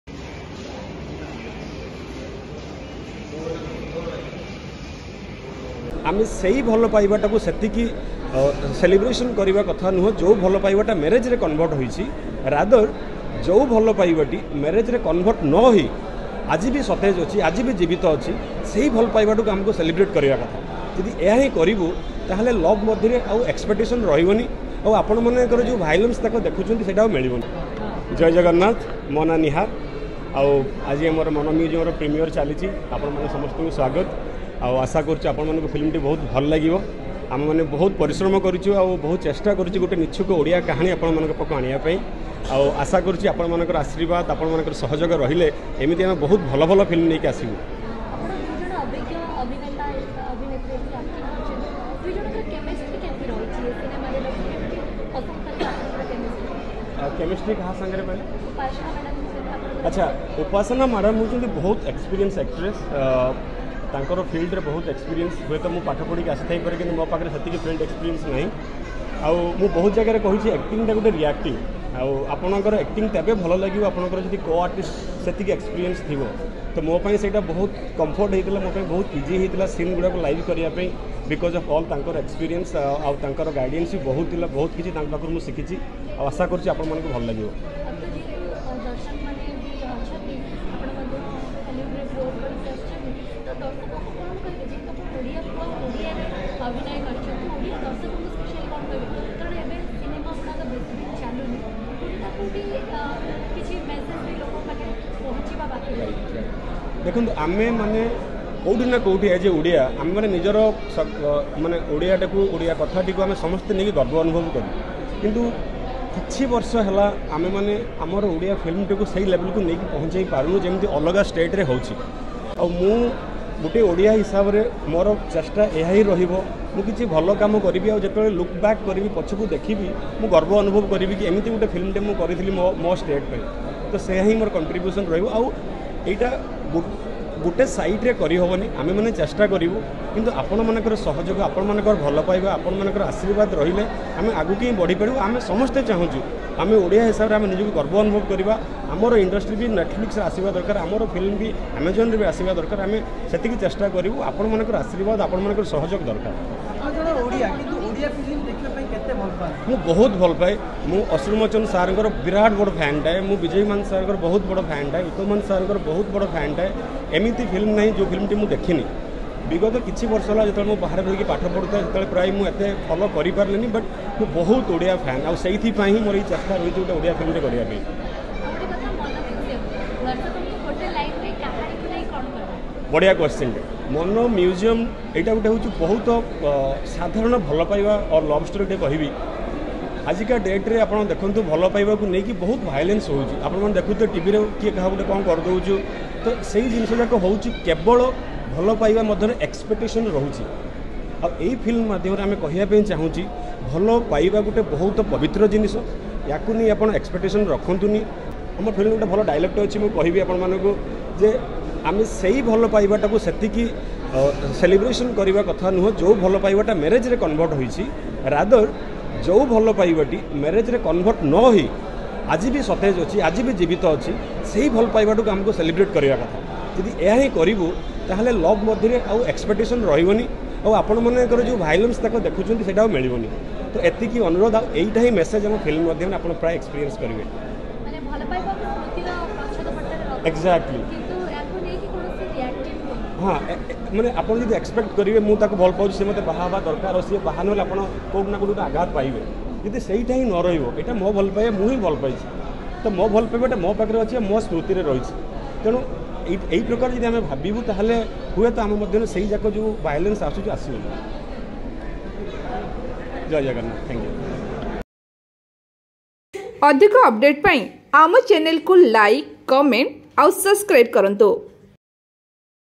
Mana Museum : Odia Movie Released | Actor Nihar's Reaction | Upasana | Animesh | Sayal | Fun Tadka
"Mana Museum" is an Odia movie released on 24 Mar, 2023. The movie is directed by Dhrubanand Panda and featured Upasna Bhadra Mohanty, Nihar Nayak in lead roles. Sayal Mukherjee & Animesh Ghosh were cast in the movie as child actors. This movie produced by Sangita Mohanty under the banner of Sangita Motion Pictures and music directed by Sarat Nayak.

Hope you guys enjoy it and love it.